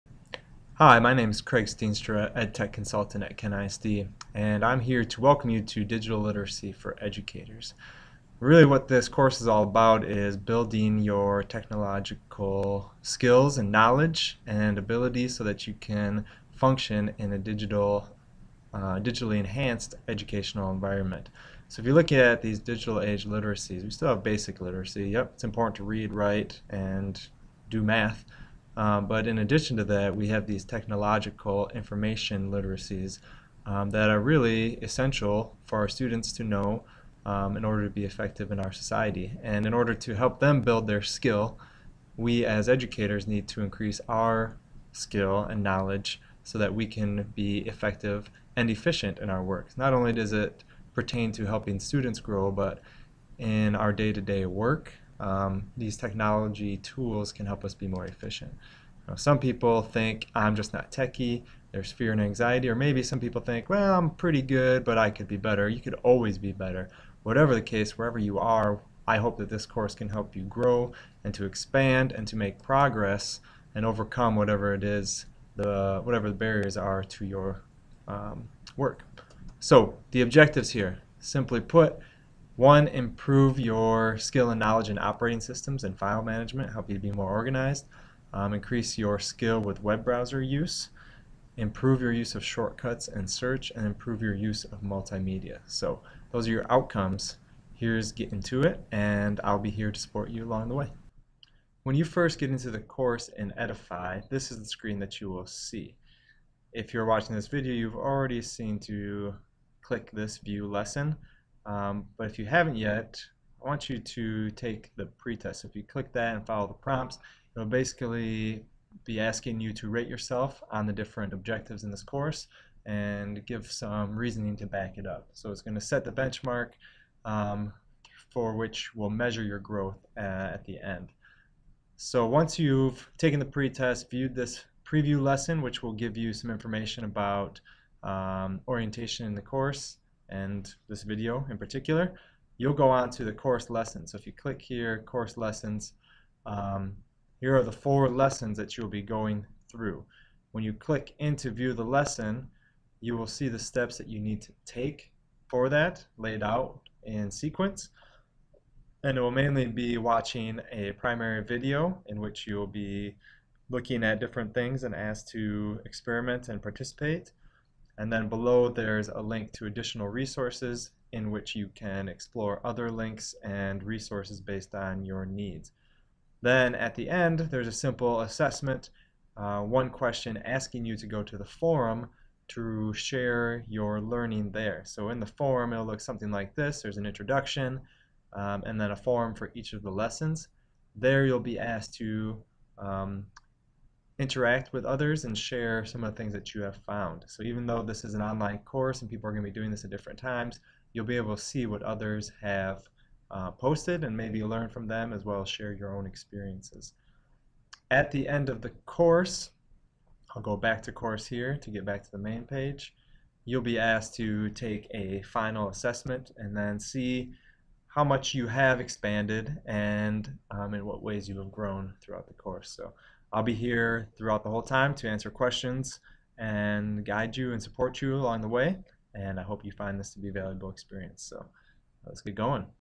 Digital Literacy for Educators Intro
An intro video to an online course on digital literacy for Educators